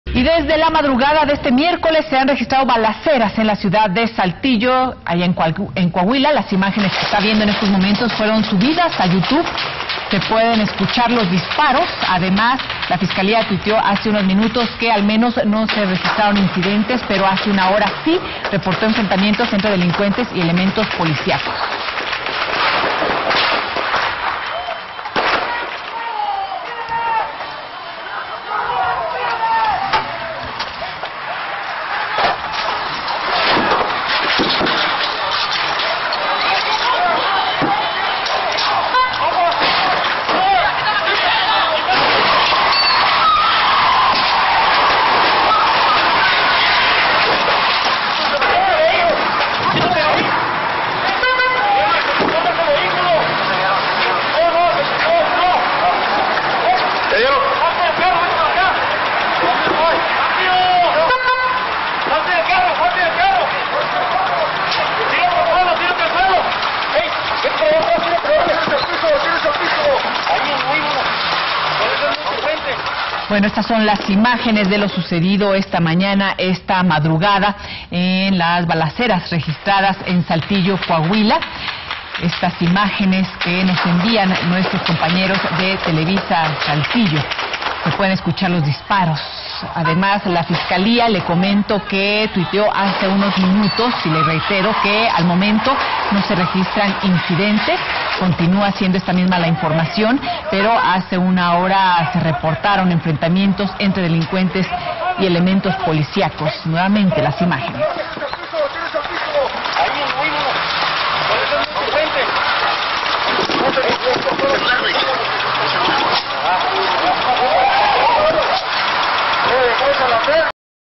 BALACERAS EN SALTILLO COAHUILA
Los habitantes de esta ciudad han vivido momentos de pánico y caos por las balaceras que se han registrado prácticamente durante todo el día en diferentes puntos de la ciudad debido a enfrentamientos entre elementos de la policía y comandos armados. Hasta el momento, extraoficialmente se ha tenido conocimiento de varios muertos, sin embargo la cifra no es precisa porque las autoridades no han proporcionado información oficial. Las balaceras en diferentes puntos de la ciudad provocaron temor entre los padres que llevaban a sus hijos a las escuelas, entre los comerciantes que abrían sus negocios y en las personas que se encontraban en los hoteles y en el Club San Isidro. De acuerdo a la información proporcionada por la Fiscalía General del Estado y de la Policía Municipal de Saltillo, así como del propio alcalde Jericó Abramo Masso, a lo largo del día se han registrado diversos enfrentamientos entre los civiles armados y los elementos de la policía. La primera balacera se reportó a las 7:20 de la mañana al norte de la ciudad por el boulevard Luis Donaldo Colosio. Minutos después se reportaron balaceras en Venustiano Carranza, a la altura de Campanares, Colosio y campestre, luego al norte de Venustiano Carranza a la altura de "De Acero". Minutos más tarde hubo balazos en Mira Sierra y luego en Venustiano Carranza a la altura de Colosio y luego en Pedro Figueroa. Cabe destacar que hubo una tregua entre las 9:00 y 9:40 de la mañana cuando las autoridades daban por controlada la situación, volviéndose a presentar un enfrentamiento en Venustiano Carranza y Colosio, más tarde en Pedro Figueroa. Después de un tiempo, las autoridades informaron de balaceras en Venustiano Carranza y Pedro Figueroa, Valdés Sánchez y Eulalio Gutiérrez, en la carretera a Los González y en Urdiñola y Nazario Ortiz. Alrededor de las 11:15 de la mañana, la Fiscalía reportó que no había balaceras, sin embargo a las 12:24 del día, la Policía de Saltillo reportó un enfrentamiento más por el rumbo de Senderos.